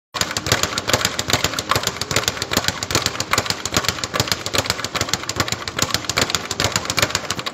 working the speed bag blind folded
Trying too work on the speed bag blind folded, have a look how i get on??